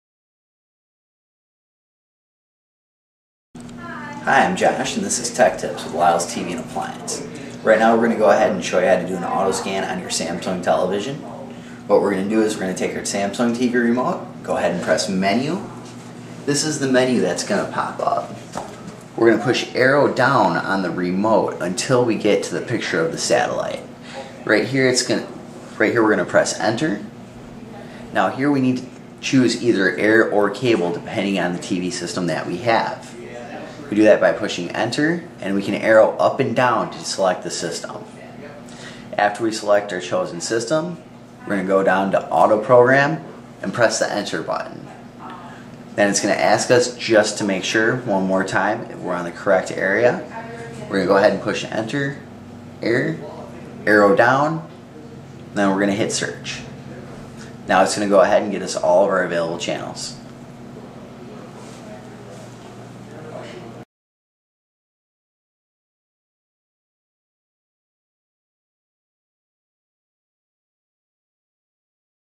How To Scan Your Samsung TV
Here is how to scan the newer TV's from Samsung that are out today.